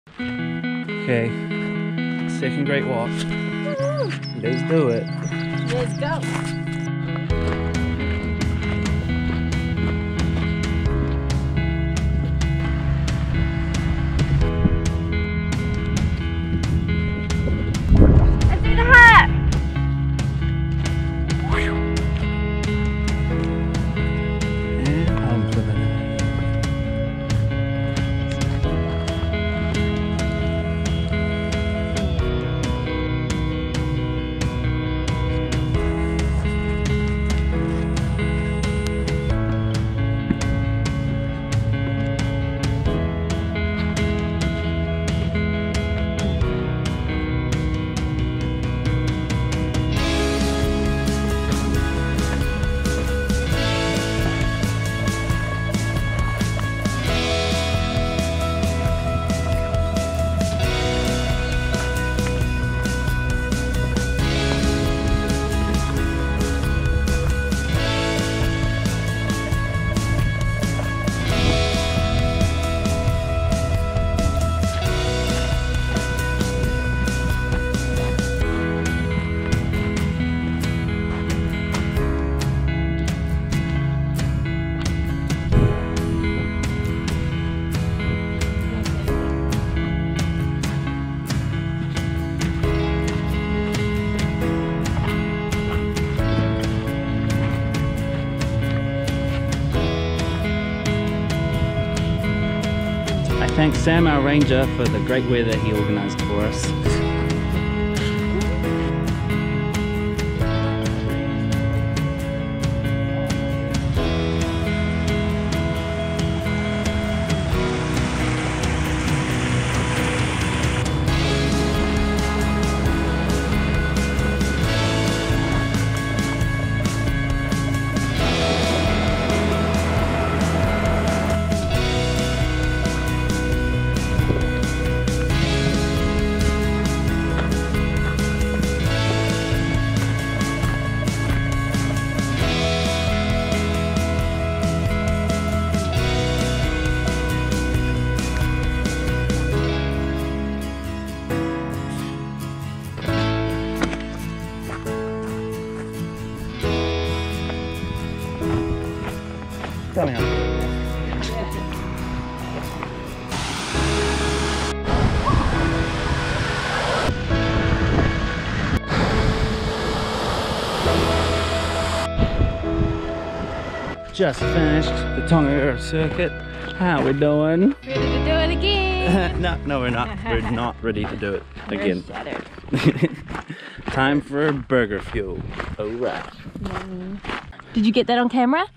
Tongariro Northern Circuit // Great Walk of New Zealand
Join the Tongariro crossing for half a day then have the National Park all to yourself for a few days!

Completed in January 2020!
Filmed by Peter Sim

Filmed on Go Pro Hero 7: Black Edition